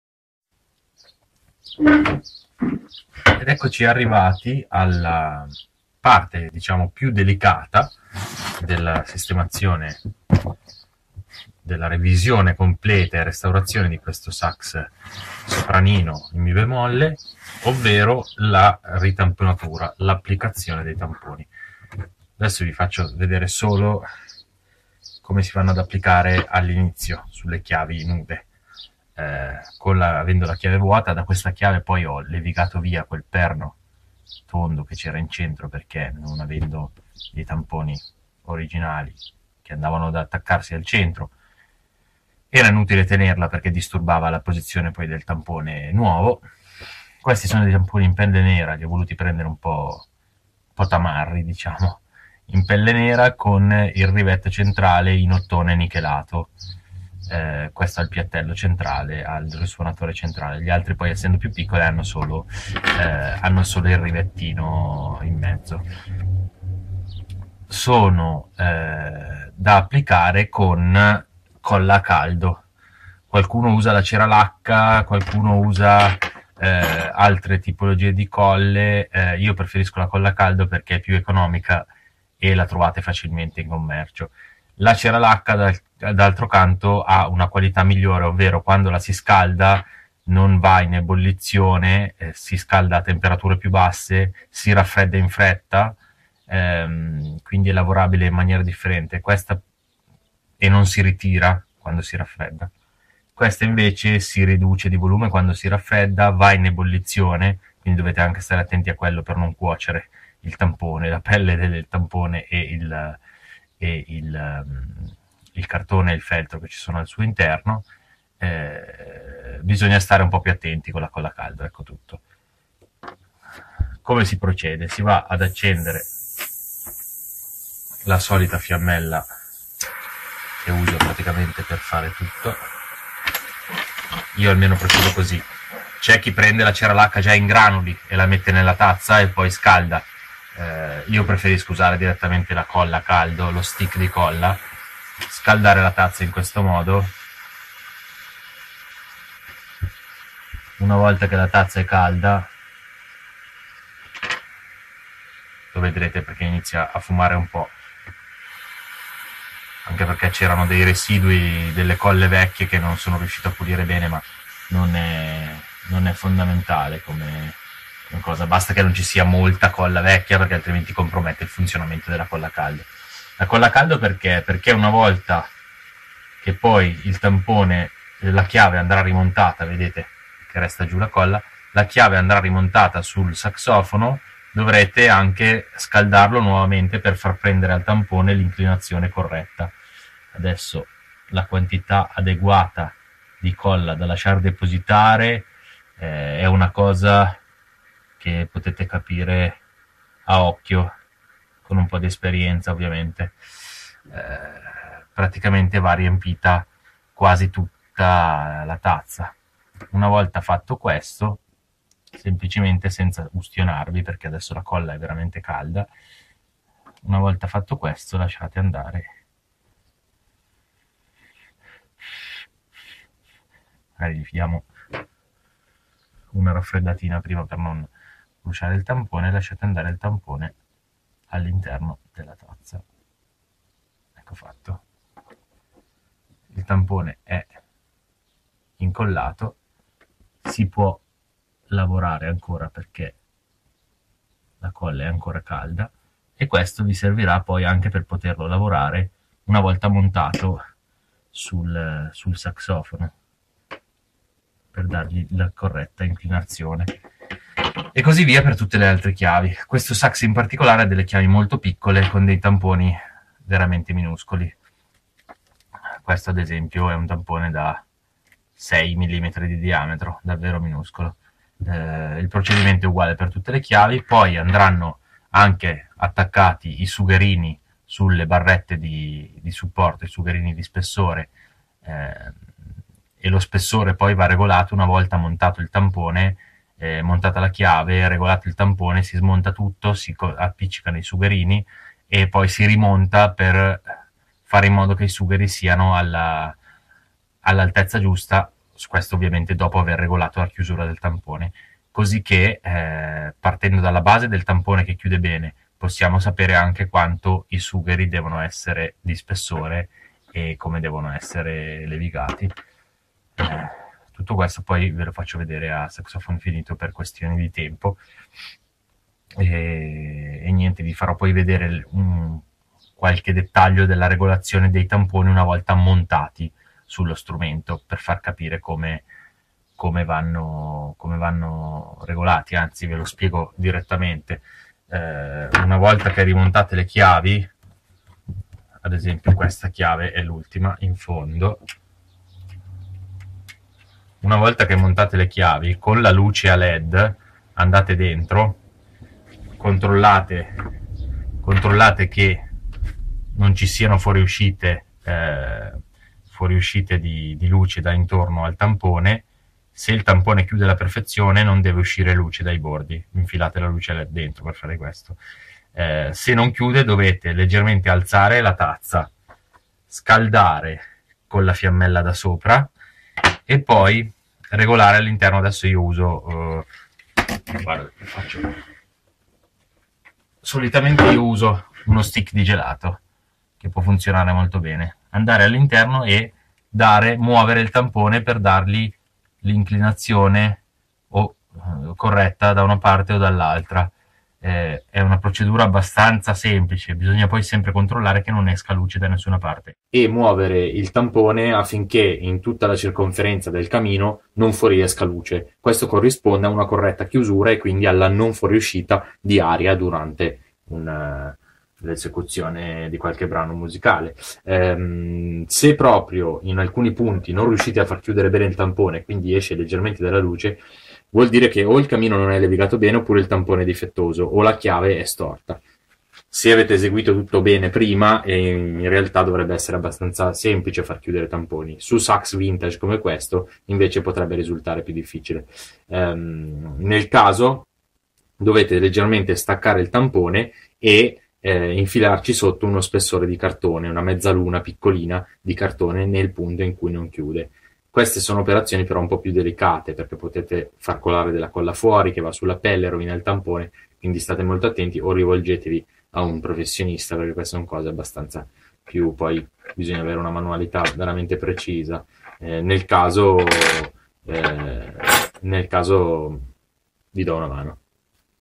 riparazione sax 5 (applicazione e regolazione tamponi) "sopranino" Mib
quinto video sulla riparazione/restauro da me eseguita su saxofono "sopranino" in mi bemolle...link agli altri video:

commentate!
gradisco molto suggerimenti ed opinioni costruttive!